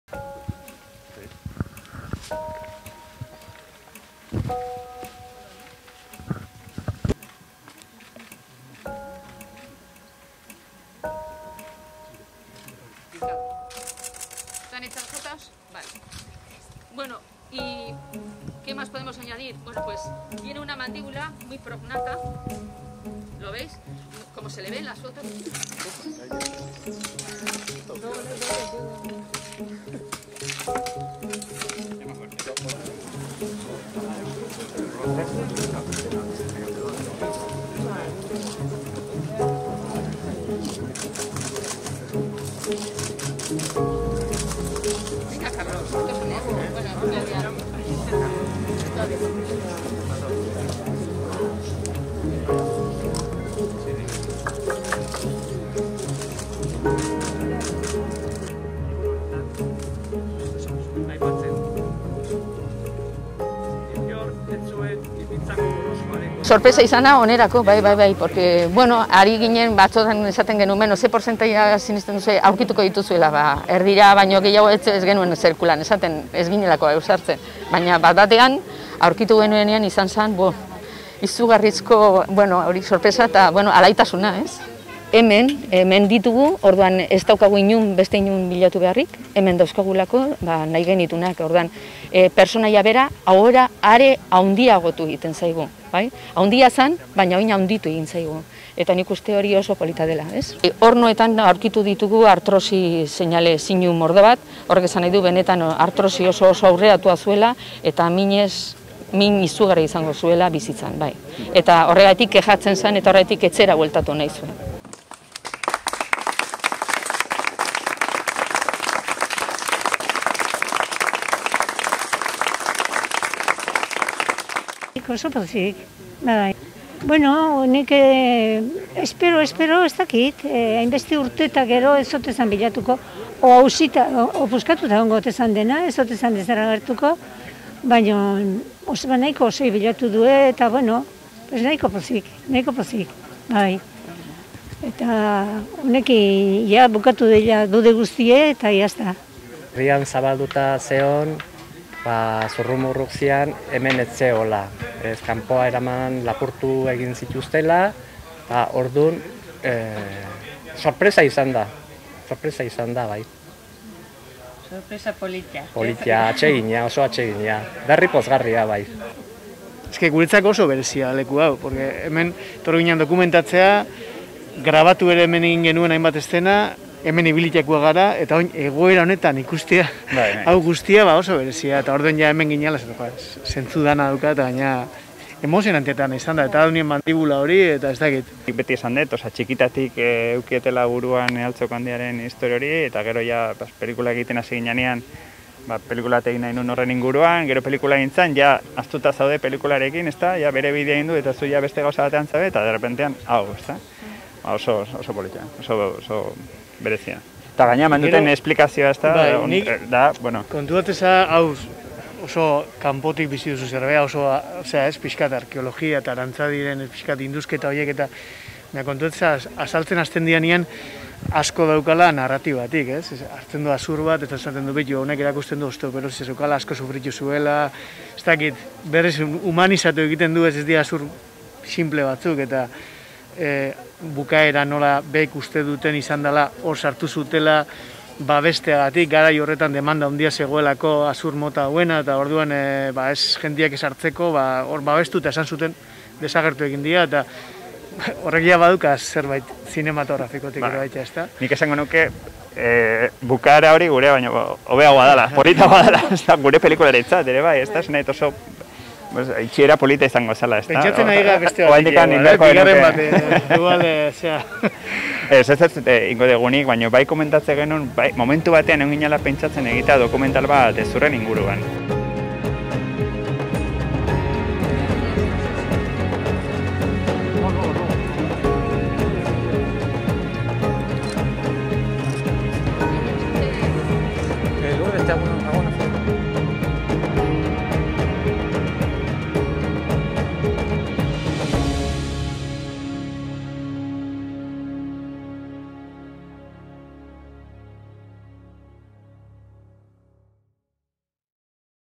Altzoko Handia-ren hezurrak aurkitu dituzte
| Migel Joakin Eleizegi Arteagaren hezurrak aurkitu ditu Aranzadi zientzia elkarteak Altzo azpiko hilerrian, duela 160 urte lurperatu zuten leku berean.